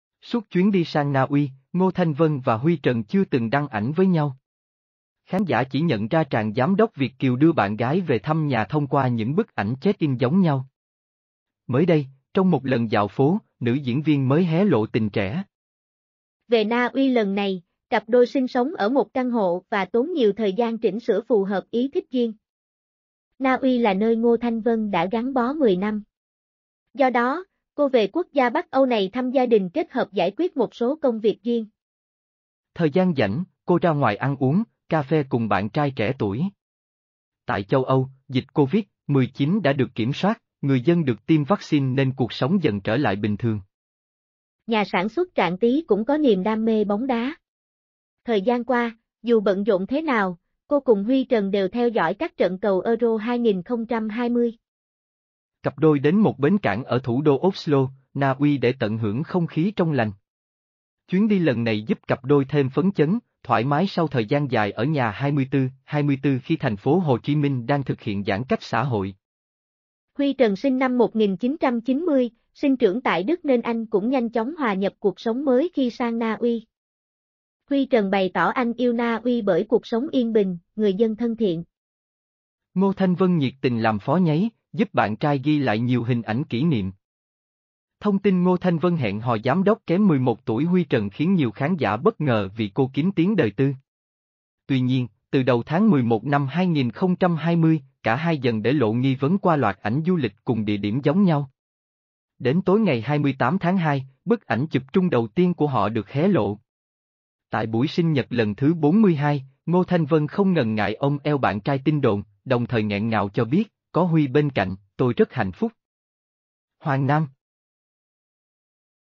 Những ngày Ngô Thanh Vân và tình trẻ ở Na Uy
Suốt chuyến đi sang Na Uy, Ngô Thanh Vân và Huy Trần chưa từng đăng ảnh với nhau. Khán giả chỉ nhận ra chàng giám đốc Việt kiều đưa bạn gái về thăm nhà thông qua những bức ảnh check-in giống nhau. Mới đây, trong một lần dạo phố, nữ diễn viên mới hé lộ tình trẻ.
Về Na Uy lần này, cặp đôi sinh sống ở một căn hộ và tốn nhiều thời gian chỉnh sửa phù hợp ý thích riêng.
Na Uy là nơi Ngô Thanh Vân đã gắn bó 10 năm. Do đó, cô về quốc gia Bắc Âu này thăm gia đình kết hợp giải quyết một số công việc riêng.
Thời gian rảnh, cô ra ngoài ăn uống, cafe cùng bạn trai trẻ tuổi. Tại châu Âu, dịch Covid-19 đã được kiểm soát, nguời dân được tiêm vaccine nên cuộc sống dần trở lại bình thường.
Nhà sản xuất Trạng Tí cũng c...